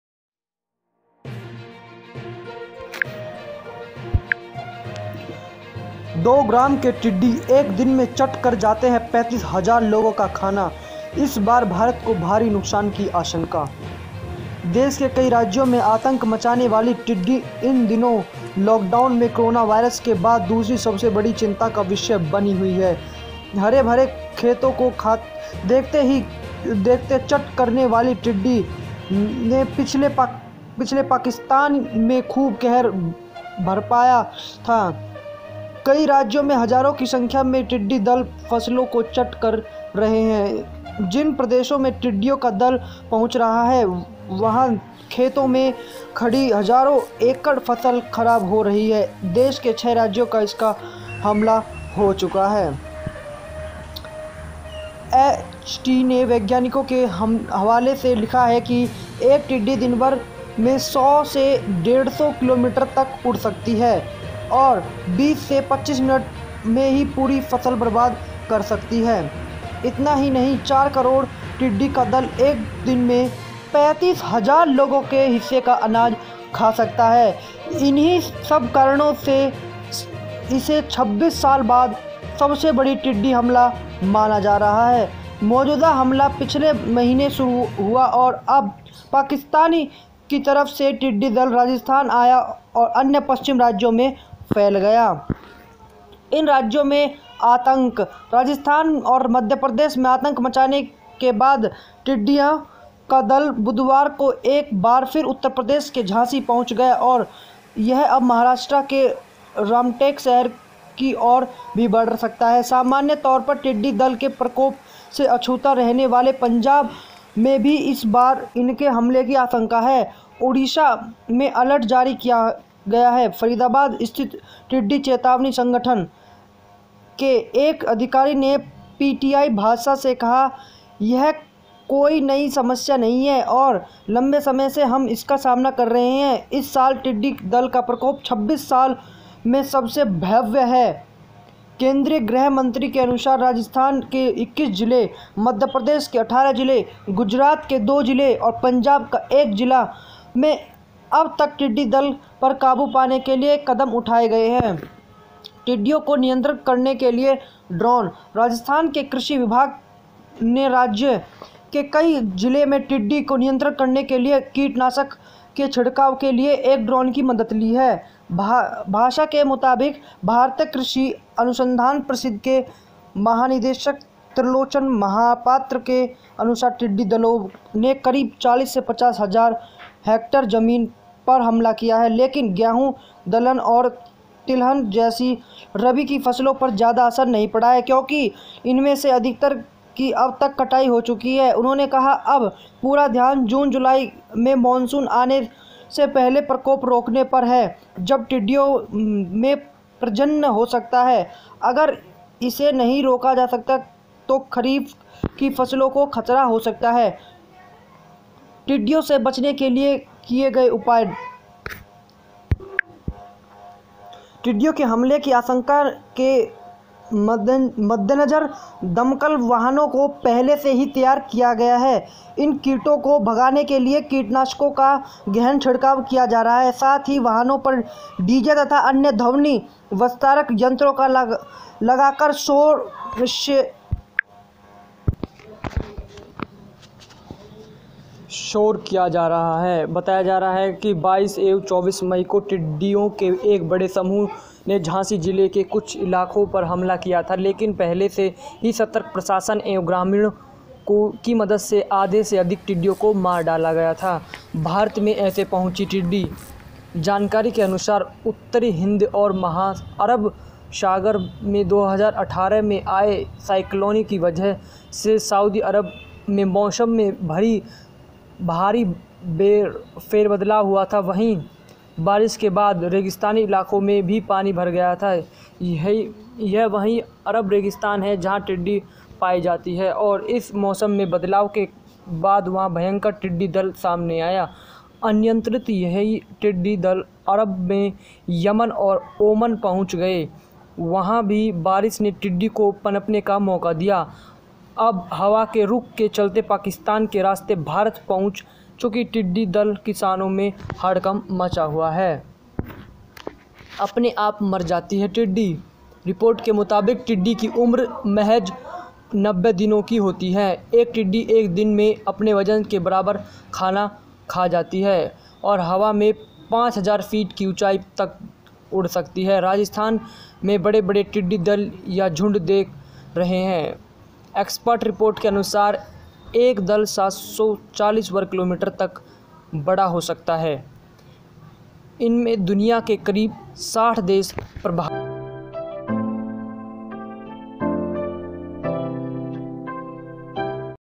दो ग्राम के टिड्डी एक दिन में चट कर जाते हैं 35 हजार लोगों का खाना, इस बार भारत को भारी नुकसान।
दो ग्राम के टिड्डी एक दिन में चट कर जाते हैं 35 हजार लोगों का खाना, इस बार भारत को भारी नुकसान की आशंका

भारत में ऐसे पहुंची टिड्‌डी

जानकारी के अनुसार उत्तरी हिंद और अरब सागर में 2018 में आए साइक्लोनों की वजह से सऊदी अरब में मौसम में भारी फेरबदल हुआ था. वहां बारिश के बाद रेगिस्तानी इलाकों में भी पानी भर गया था. यह वहीं अरब रेगिस्तान है जहां टिड्‌डी पायी जाती थी और इस मौसम में बदलाव के बाद वहां भयंकर टिड्‌डी दल सामने आए. अनियंत्रित यही टिड्‌डी दल अरब से यमन और ओमान पहुंचे गए. वहां भी बारिश ने टिड्‌डी को पनपने का मौका दिया. अब हवा के रूख के चलते पाकिस्तान के रास्ते भारत पहुंच चुकी टिड्‌डी दल किसानों में हड़कंप मचाए हुए है.